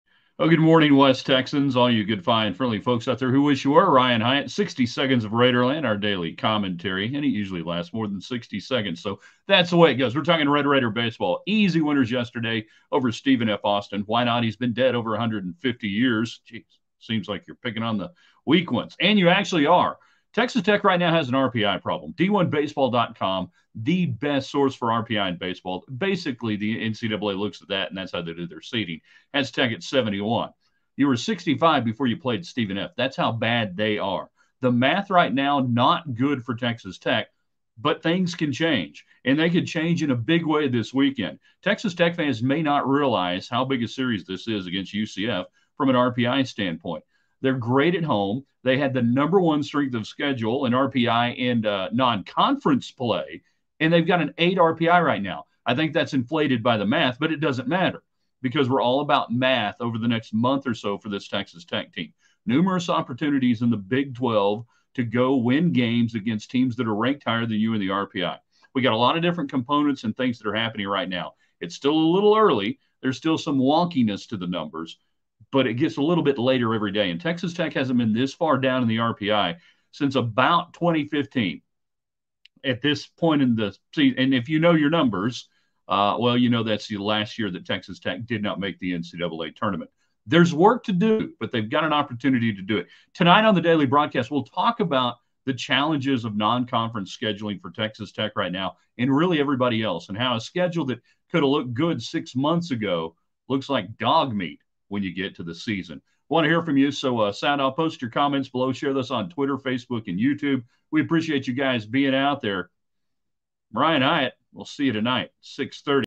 3 27 24 60 Seconds of Raiderland Texas Tech Baseball And The RPI Problem, Can It Be Fixed
TexasTech Baseball has more wins than anyone in the Big12 and they also have an RPI problem this season. Can it be fixed? Good news. It can. A quick look at some collegebaseball and more of ya !